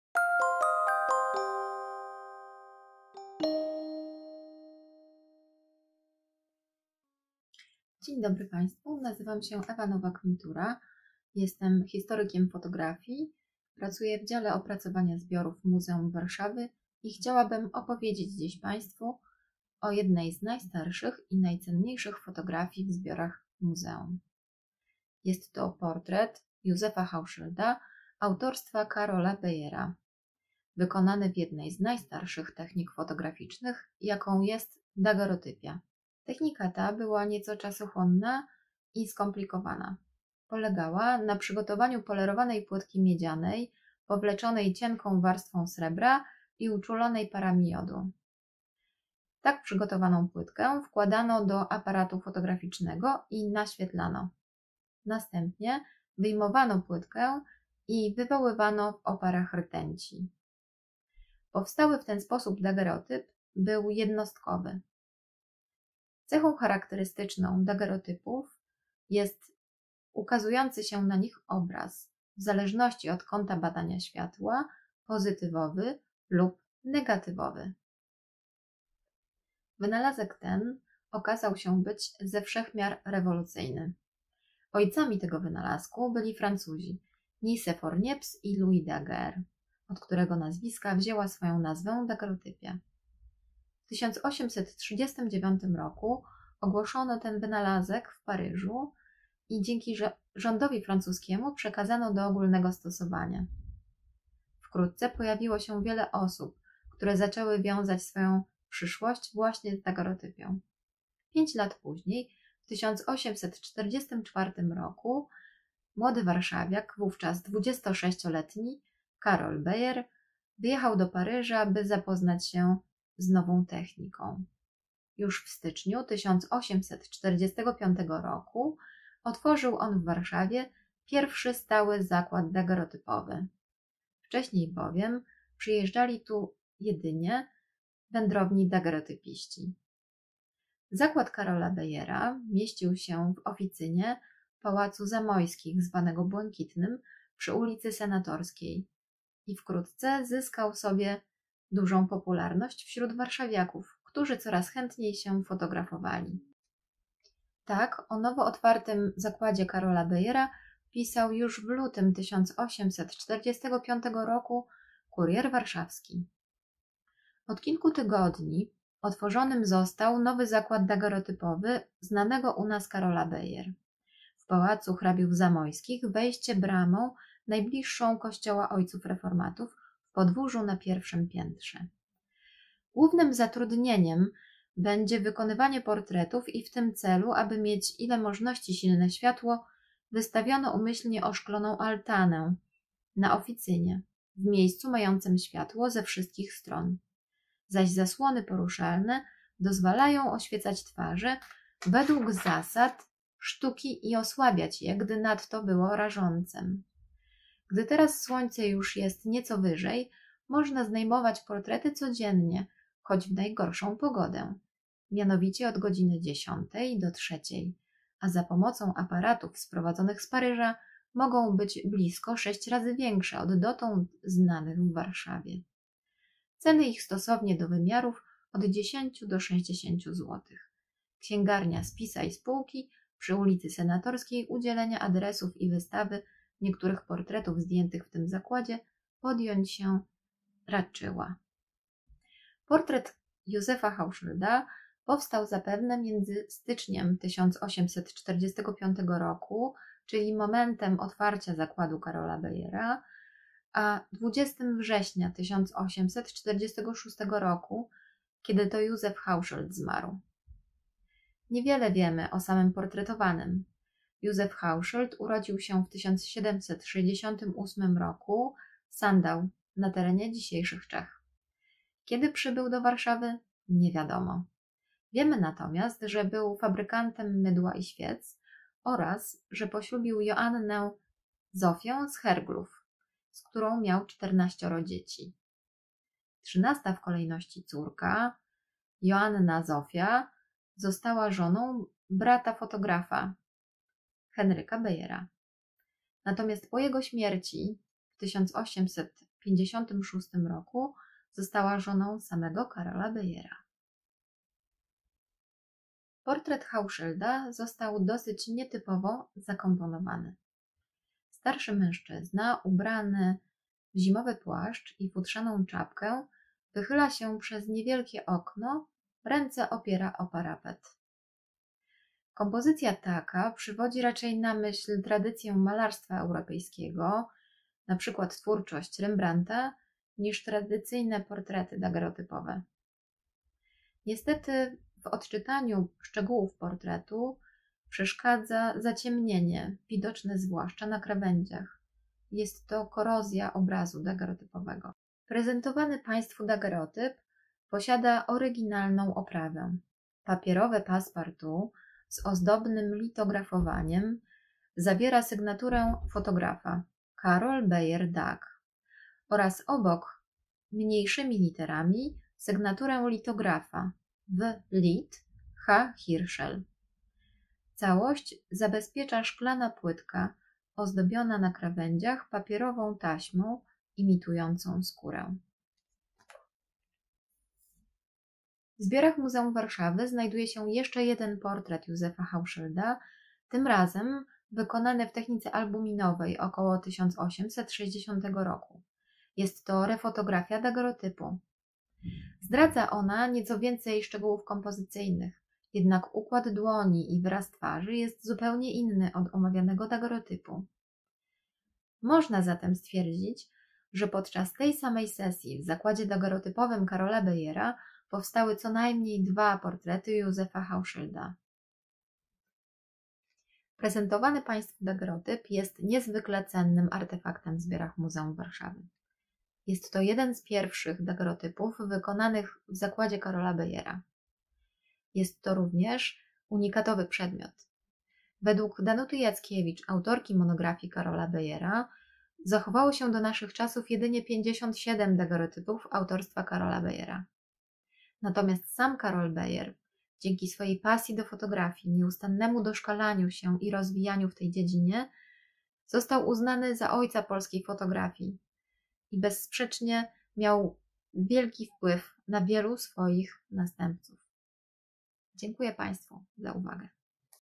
Fotografia Karola Beyera i dagerotypia
Ewa Nowak-Mitura z Działu Opracowania Zbiorów opowiada o fotografii wykonanej w jednej z najstarszych technik – dagerotypii.

Muzeum Warszawy
Rynek Starego Miasta 28–42, 00–272 Warszawa